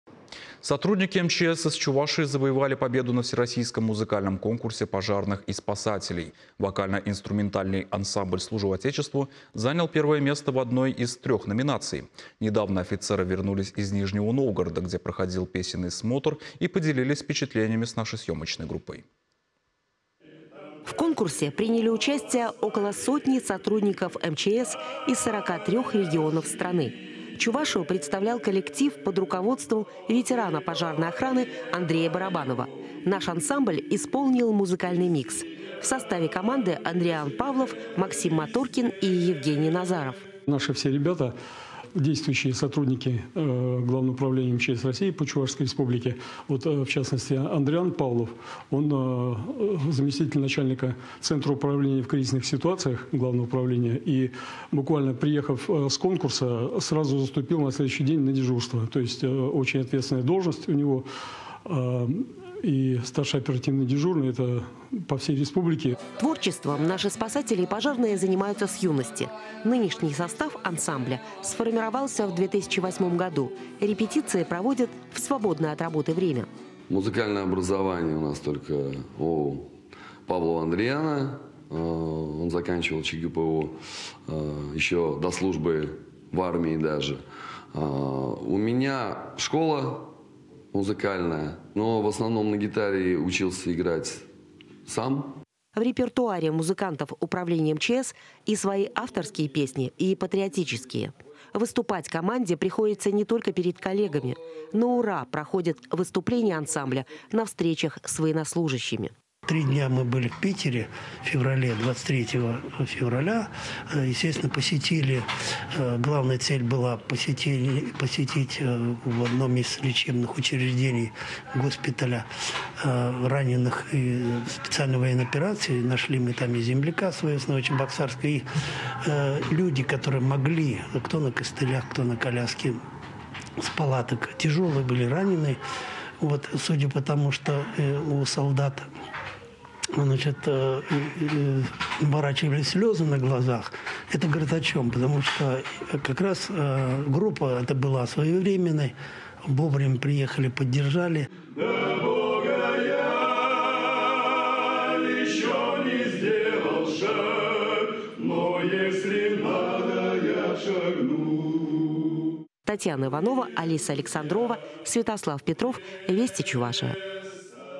Спасатели из Чувашии завоевали победу во Всероссийском музыкальном конкурсе
Сотрудники МЧС из Чувашии завоевали победу во Всероссийском музыкальном конкурсе пожарных и спасателей.